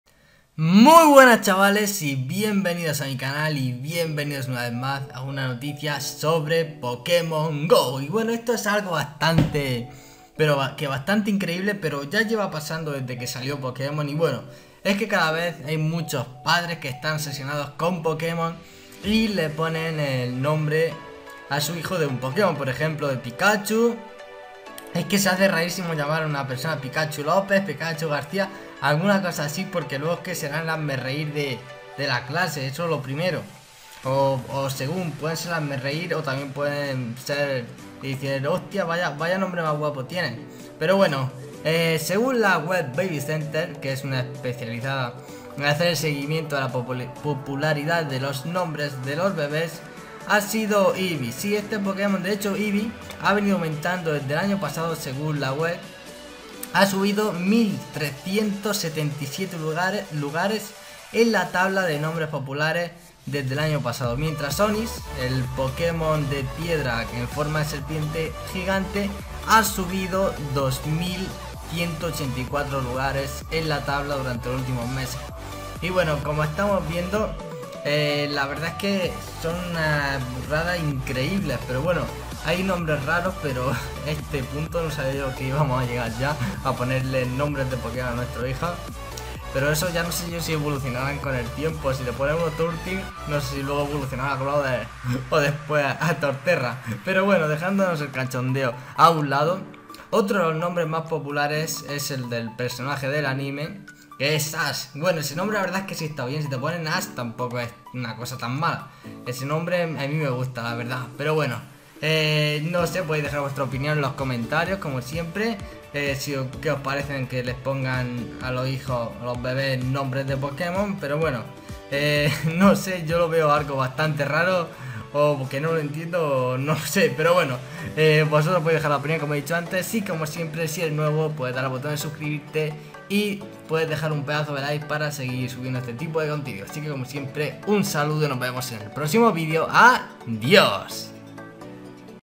Aumentan los bebés con nombres de Pokémon | Pokémon GO
Cada vez más niños tienen nombre de pokémon por culpa de padres obsesionados con Pokémon Go | Aumentan los bebés con nombres de Pokémon | Pokémon GO